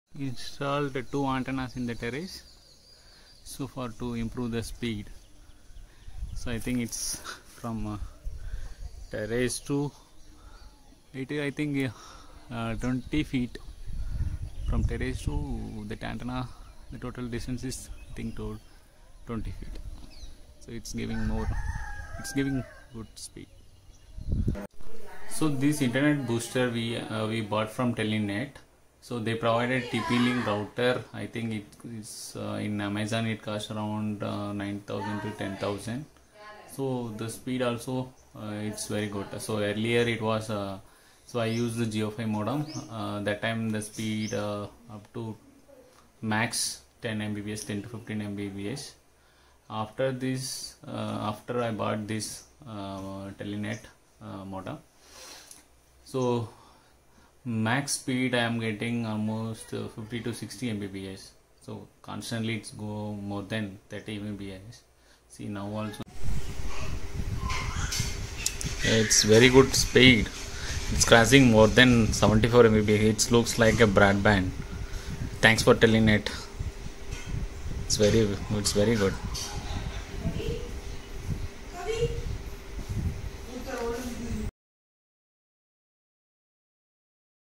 Internet Speed Booster 100% Working - 12 dBi LPDA Customer Review Speed Increased from 15 to 65 Mbps
Product - LPDA Antenna, TP-Link MR600 Router, LMR400 Coaxial Cable
Company - Teleten

Seller phone number - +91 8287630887

This Video was shared by one of our happy customers from Ramasamy Puram, Tamil Nadu.  We have used Two 12 dBi High Gain antenna with 15 meter LMR 400 Coaxial Cable and TP-Link MR 600 Router.
Clearly you can see the speed significantly increased from 15 to 65 mbps.